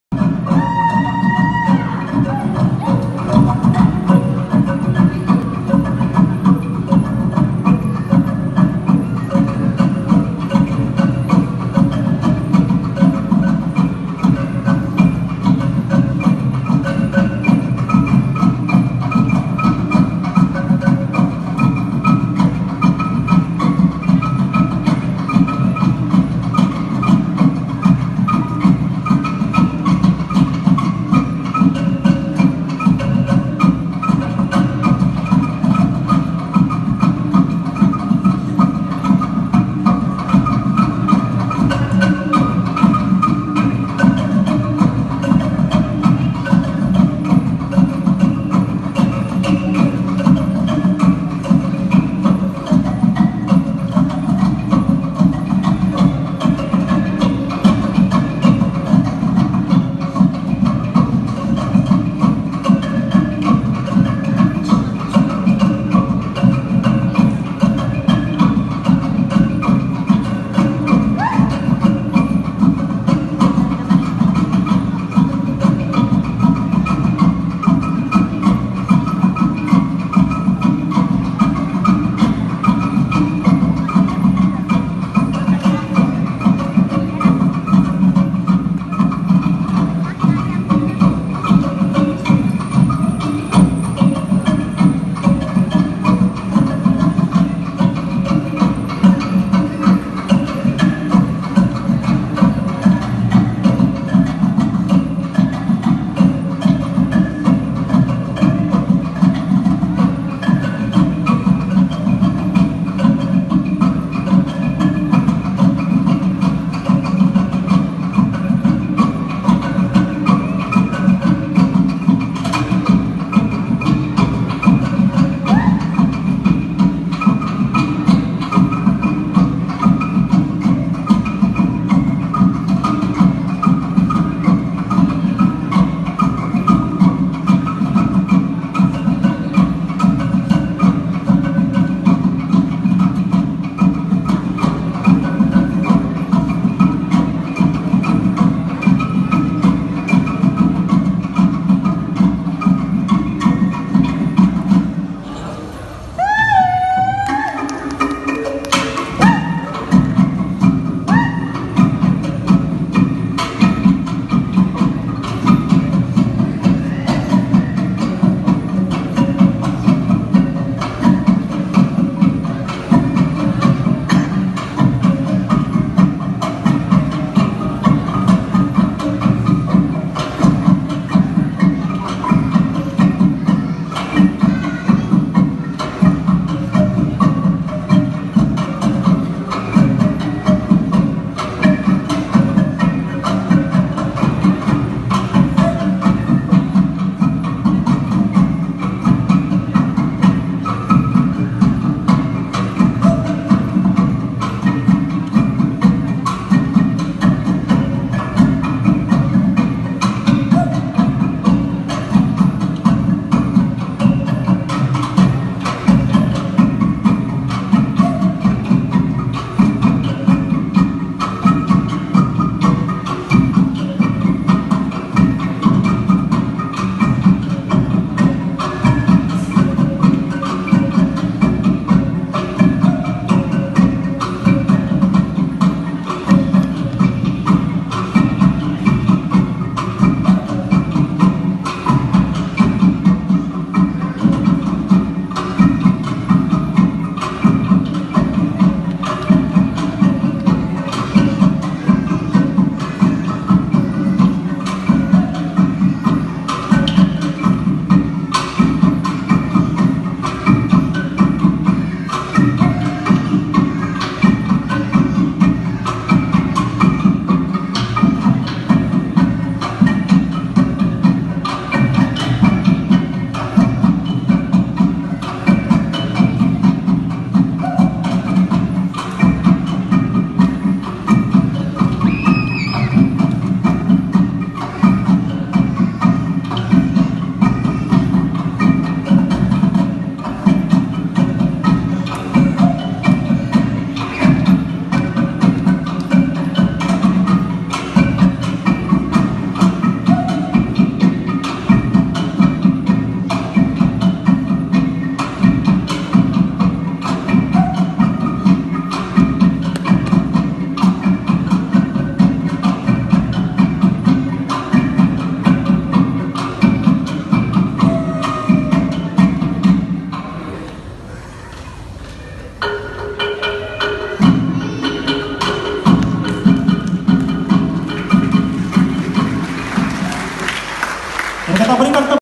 Tarian titikas dari suku kaum sungai kinabatangan
(Tarian titikas)

Tarian ini berasal dari suku kaum sungai kinabatangan di Sabah, Malaysia